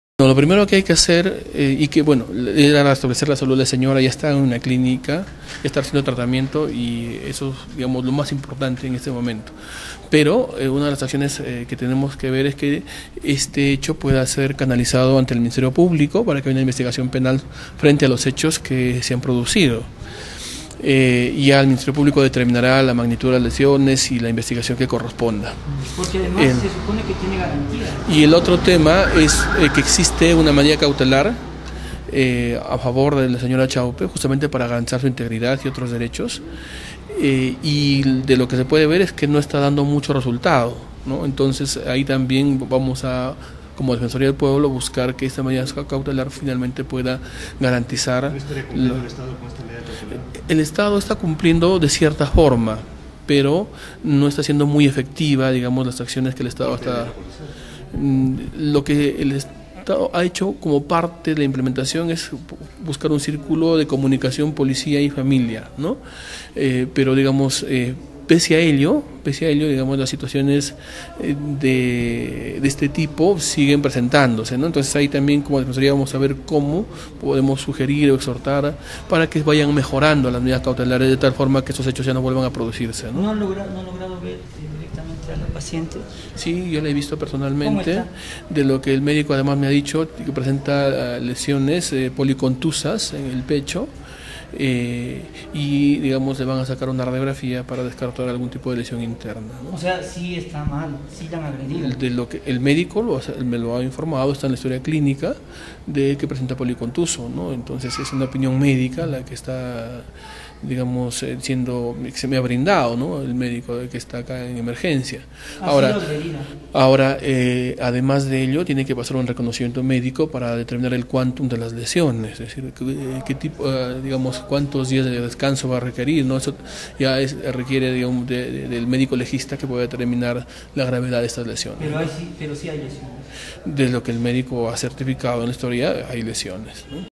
Representante de la Defensoría del Pueblo sobre agresión a Máxima Acuña
Un representante de la Defensoría del Pueblo se pronunció sobre el atentad que sufrió Máxima Acuña el pasado domingo 18 de setiembre de 2016 en su predio ubicado en tragadero grande, en Sorochuco, Celendín, Cajamarca.